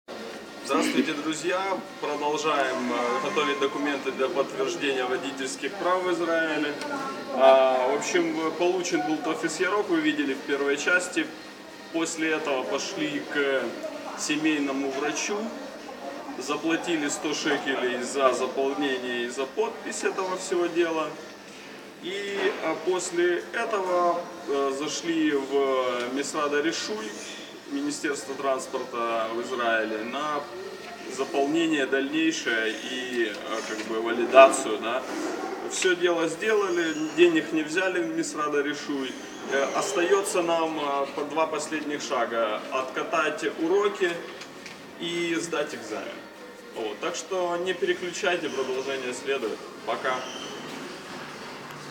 Подтверждение водительских прав в Израиле Part 2/
Подписываем тофес ярок у семейного врача и в мисрад аришуй.